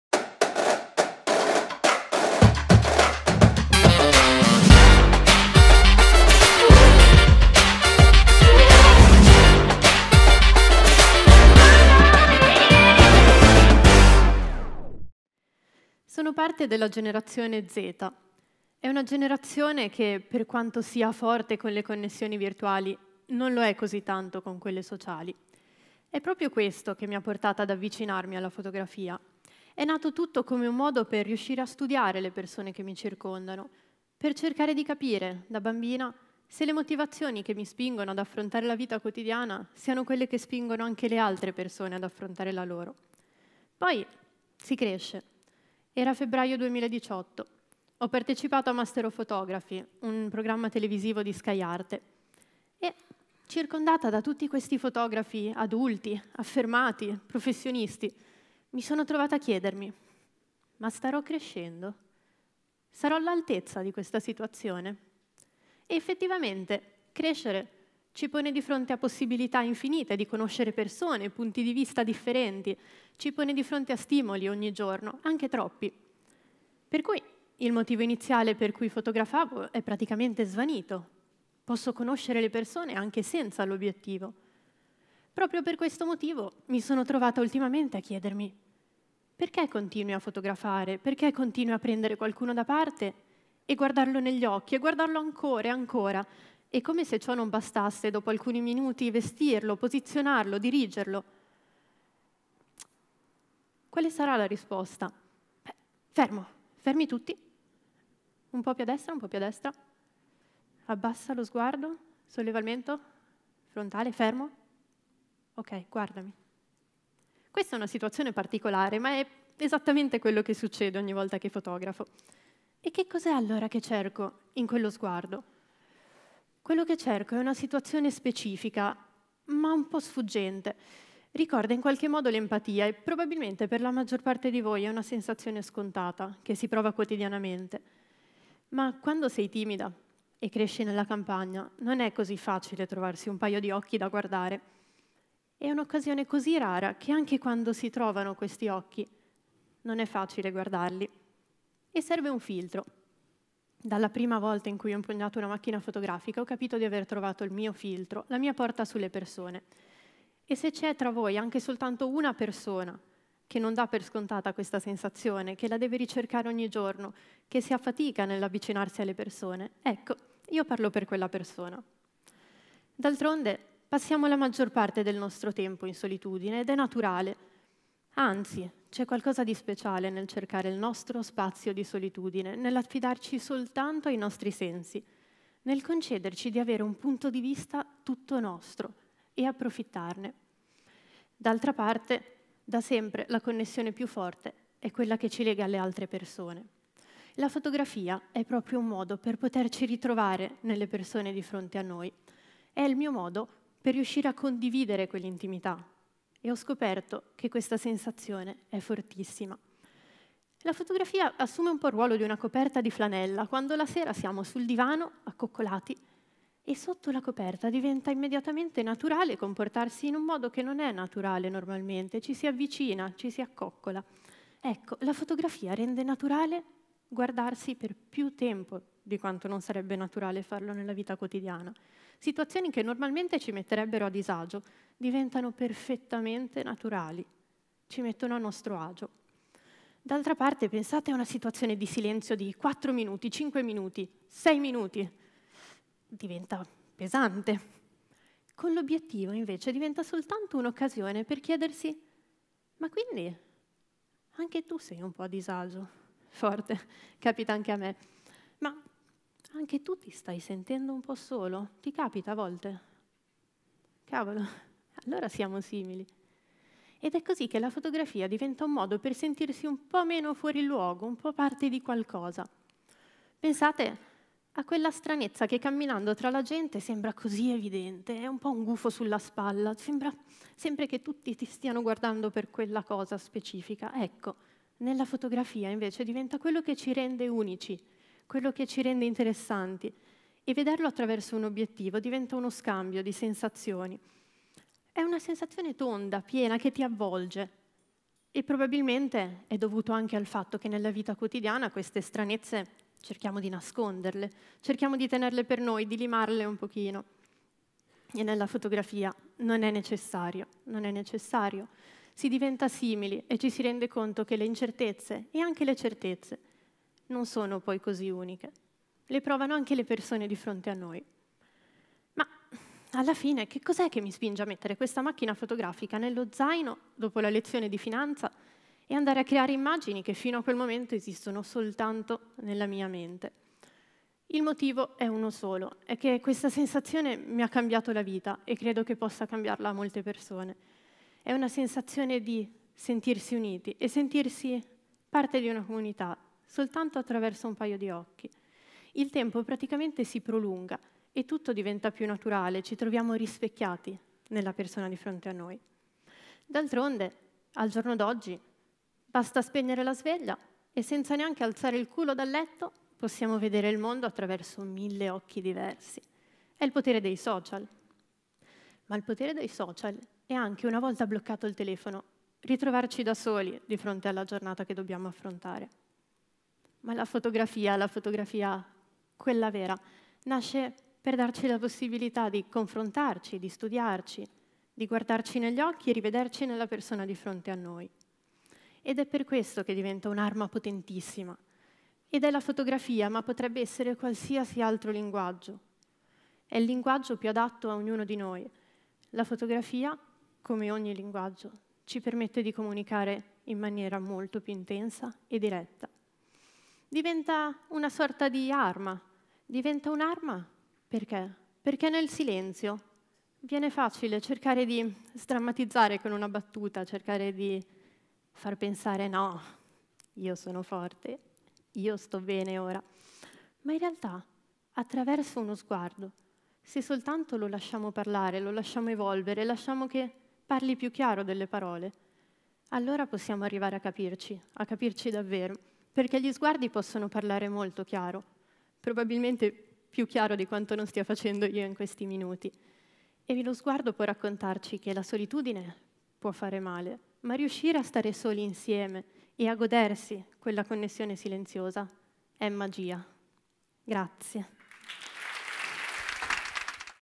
Può uno sguardo avvicinarci? | Federica Belli | TEDxCastelfrancoVeneto
Attraverso la fotografia Federica può essere se stessa e incrociando gli sguardi delle persone che fotografa può entrarne direttamente a contatto. Scopre la fotografia durante gli studi al Liceo Scientifico. Nel 2017 si trasferisce a Milano per approfondire l’esperienza fotografica, allo stesso tempo frequenta il corso di Economics & Management presso l’Università Bocconi. Nel corso dello stesso anno partecipa a Master of Photography, come partecipante più giovane. Uscendone vincitrice, ha modo di lavorare a fianco del fotografo Oliviero Toscani presso Fabrica. Nel giugno 2019 trascorre alcuni mesi a New York collaborando con il fotografo ritrattista Chris Buck e presso la rivista Musée Magazine, rivolta alla fotografia Fine Art This talk was given at a TEDx event using the TED conference format but independently organized by a local community.